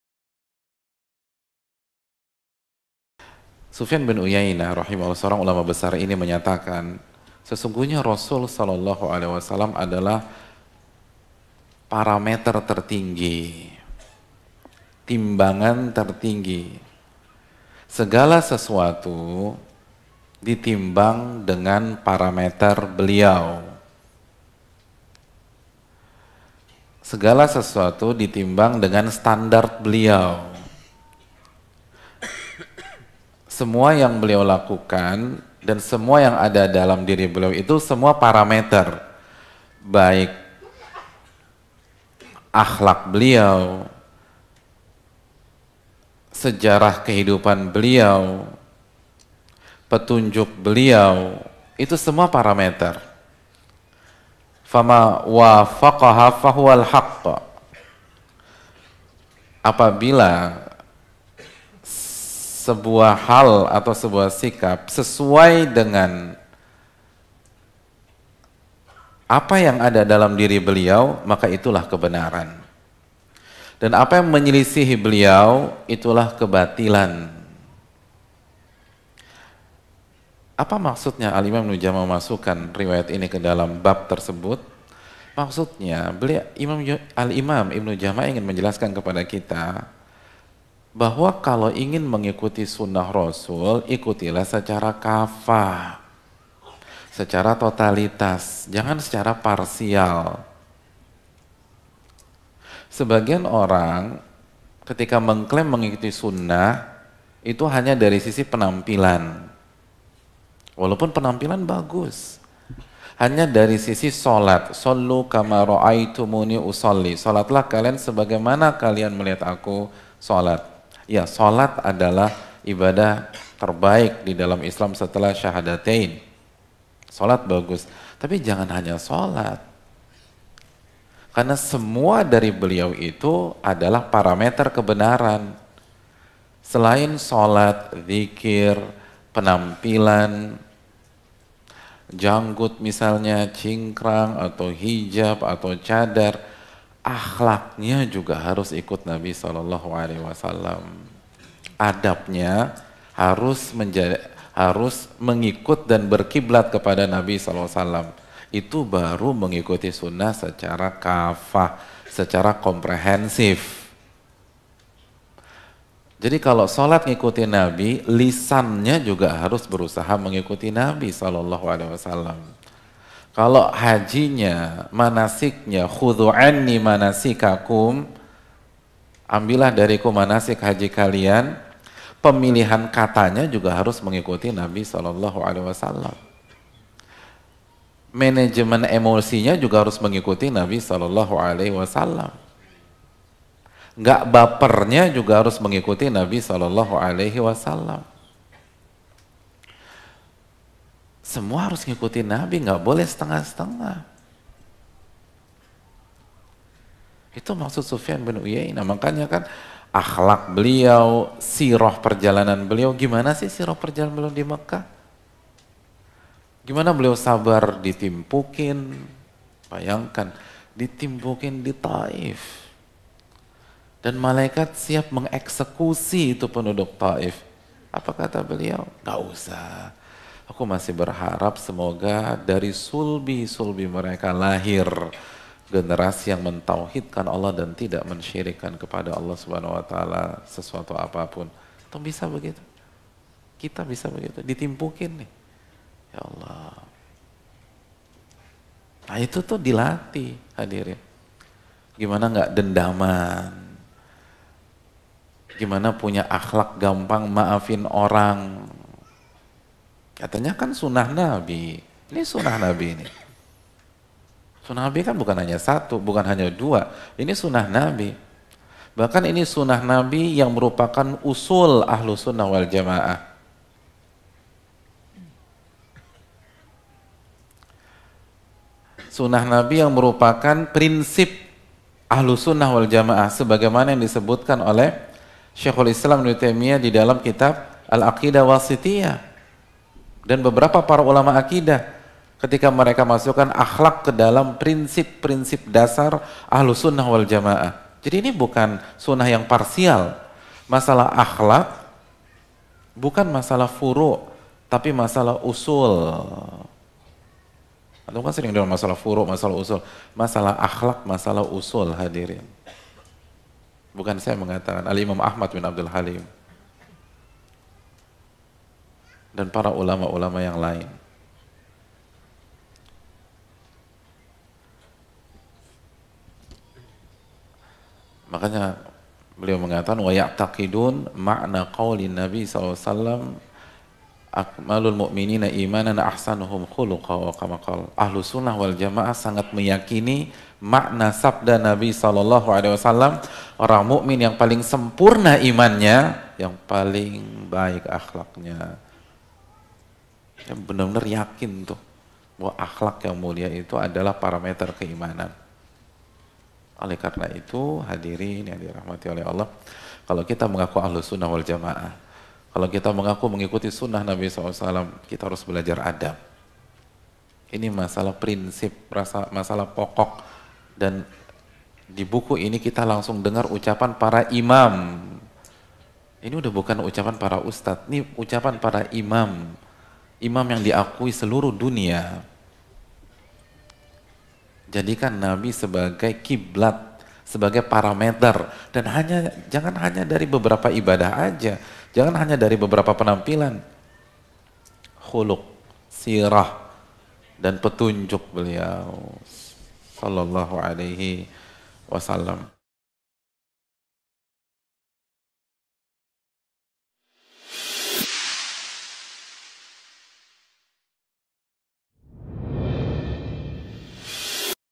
MARI MENGIKUTI SUNNAH NABI ﷺ SECARA KAFFAH (video pendek)
MARI MENGIKUTI SUNNAH NABI ﷺ SECARA KAFFAH

Video pendek diambil dari kajian:
"ULAMA PUN MEMPELAJARI ADAB"

Facebook, Instagram, YouTube, Telegram, SoundCloud: @muhammadnuzuldzikri